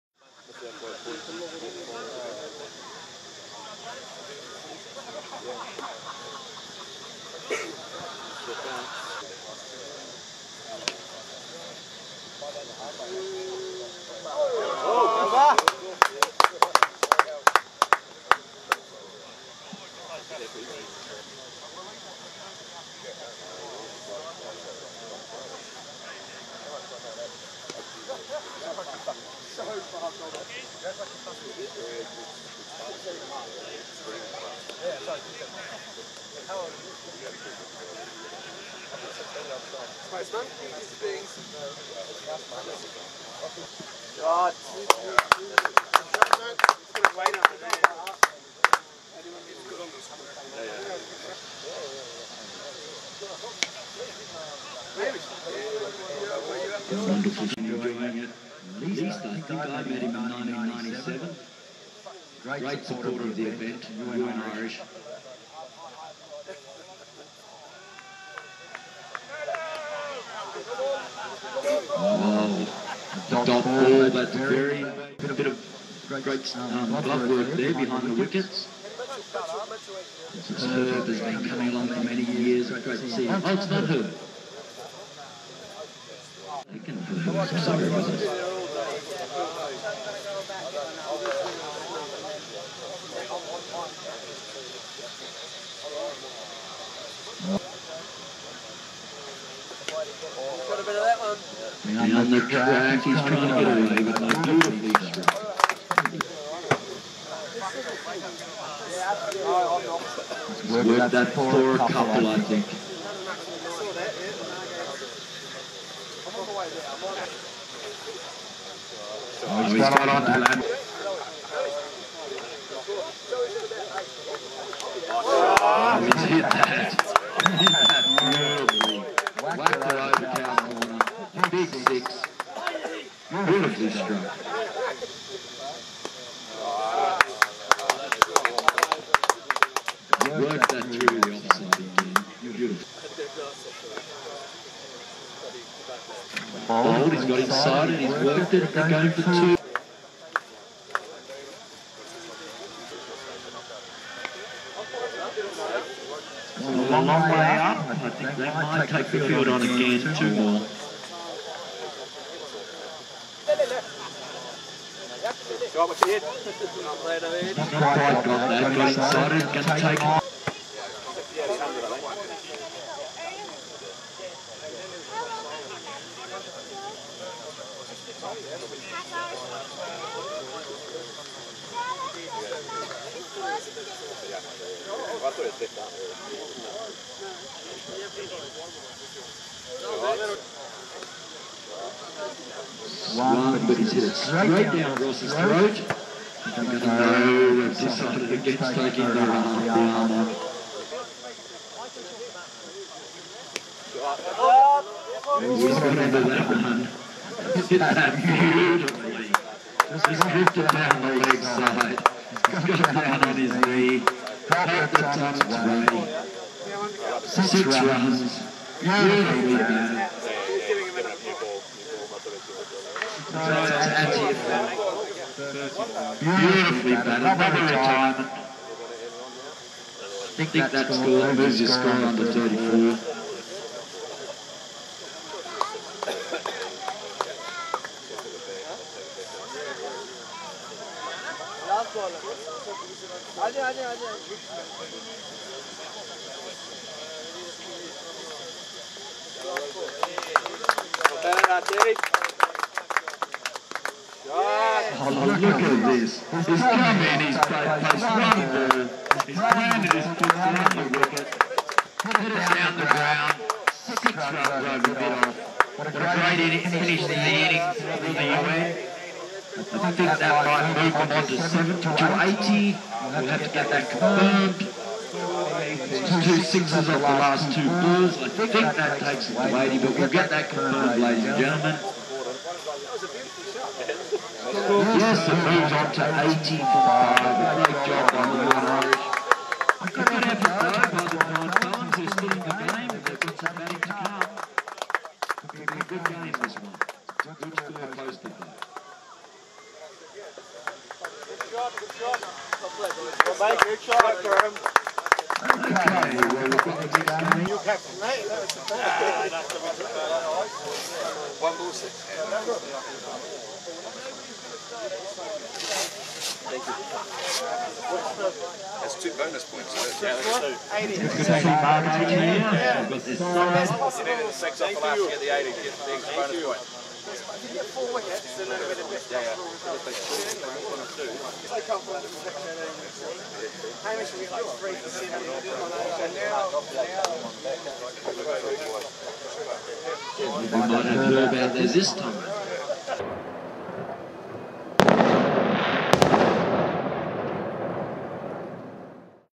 Chiang Mai International Cricket Sixes with UN Irish Pub
Day one of the sixes with UN Irish Pub playing Divine Felons.